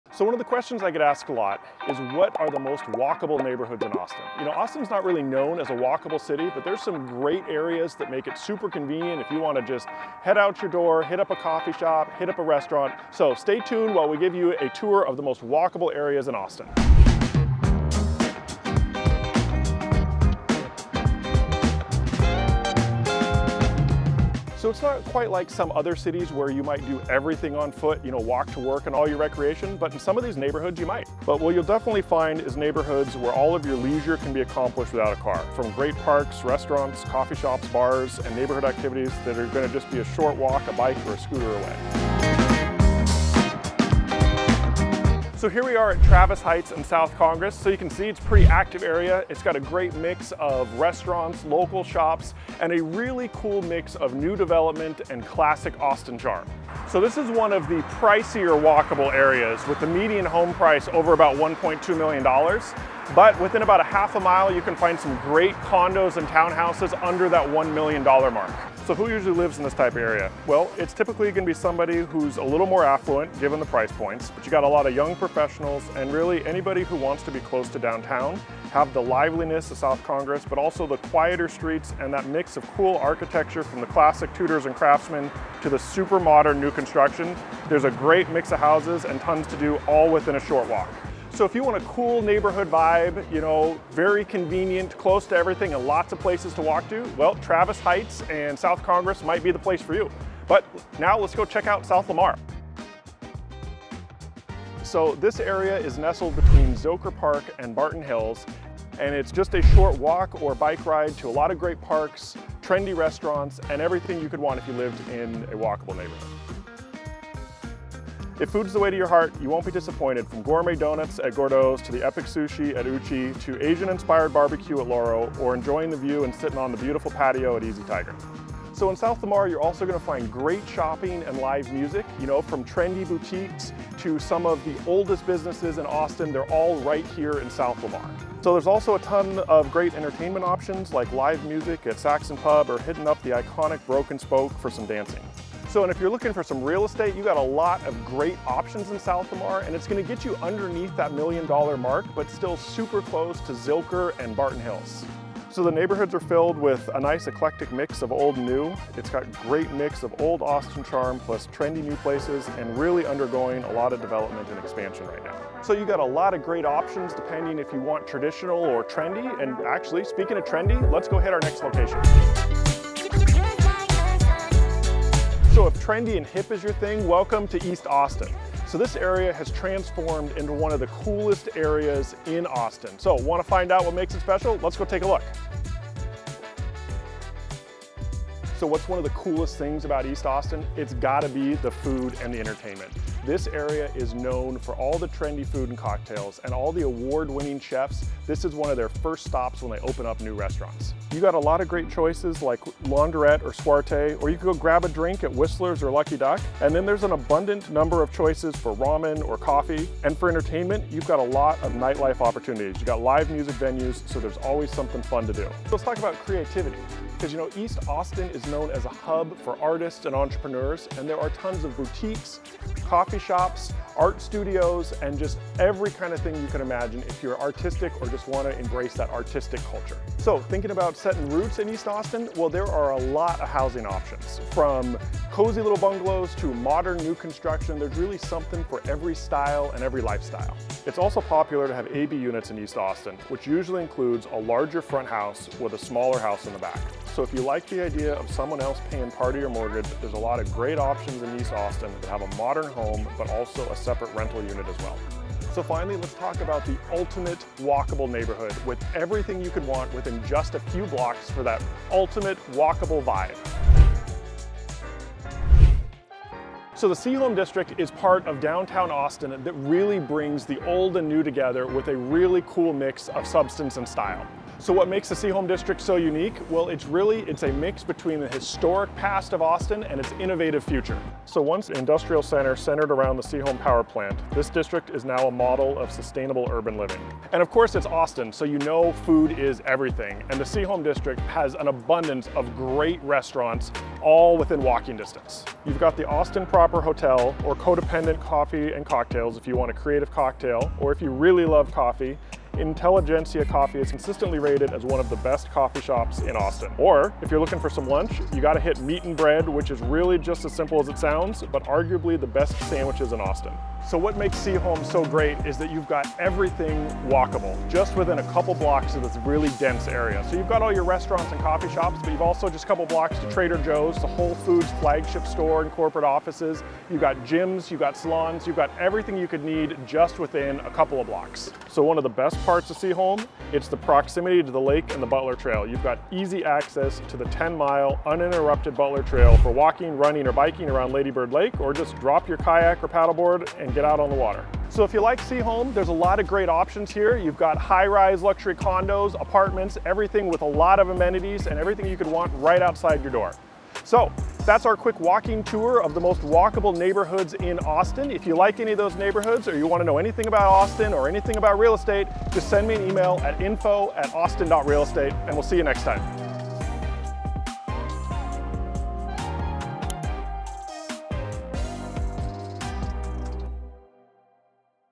Uncover the Ultimate Austin Neighborhoods
🚶‍♂️ What are Austin's Most Walkable Neighborhoods? 🚶‍♀️
Wondering which neighborhoods are the most walkable in Austin, Texas? In this video, we'll discuss the best Austin neighborhoods to explore on foot, with great restaurants, shops, and activities nearby. Whether you're a local or just visiting, these are the top spots to check out for a stroll in Austin!

Want to chat about it? Let's talk!

🏡 In this video, we'll cover:
0:00 - Introduction
0:24 - Austin's Walkable Neighborhoods

0:45 - Travis Heights and South Congress: Discover the vibrant mix of restaurants, local shops, and unique architecture in this lively neighborhood. While it may come with a higher price tag, it offers a range of housing options.

1:41 - South Lamar: Explore the fantastic location near Zilker Park and Barton Springs, with a diverse range of dining, shopping, and entertainment options. Plus, find out how it fits into a more affordable budget.

2:50 - East Austin: Immerse yourself in the cultural melting pot of East Austin, where history meets modernity. From trendy food and cocktails to art galleries and nature escapes, it's a vibrant community with housing choices for all.

4:23 - Seaholm District: Dive into the heart of downtown Austin, where historic charm meets sustainable urban living. Enjoy a diverse culinary scene and proximity to Lady Bird Lake's trails and water activities.

5:53 - Recap 🏙️ Whether you're new to Austin or a local looking for a change of scenery, these walkable neighborhoods offer convenience and a unique lifestyle.

🏘️ Thinking of making a move? We'll also provide insights into housing options in each area.

📩 Have questions or need more information about Austin real estate?